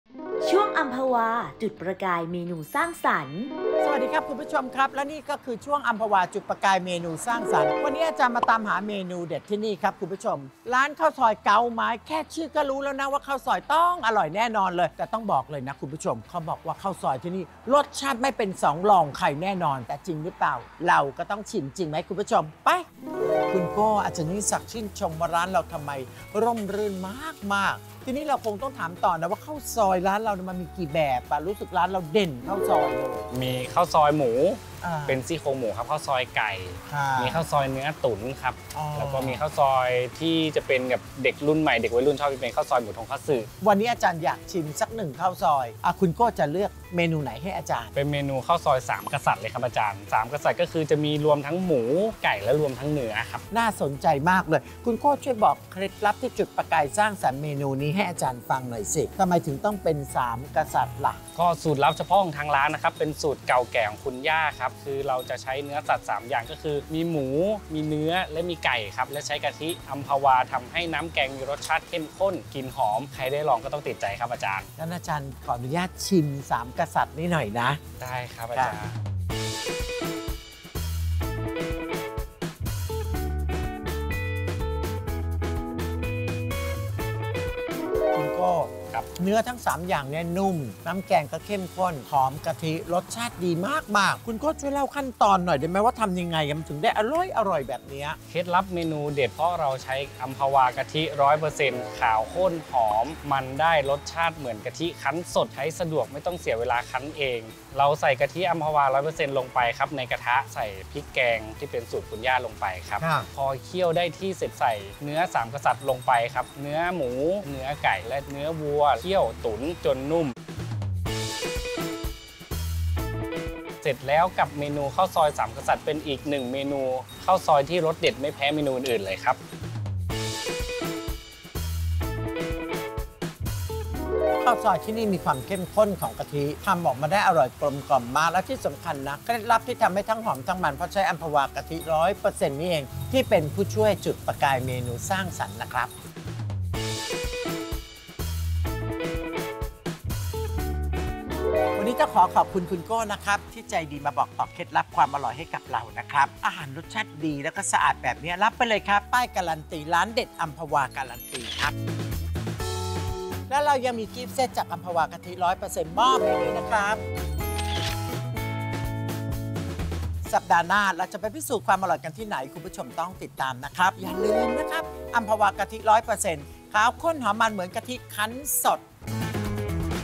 อาจารย์ยิ่งศักดิ์ ข้าวซอยสามกษัตริย์ อัมพวาจุดประกายเมนูสร้างสรรค์ | คุกกิ้งกูรู
อัมพวาจุดประกายเมนูสร้างสรรค์ ตามหาร้านเด็ดร้านดัง เพื่อรังสรรค์เมนูความอร่อยจากกะทิอัมพวา กะทิ 100 % ขาว ข้น หอม มัน เหมือนกะทิคั้นสด

▲Line @yingsakfood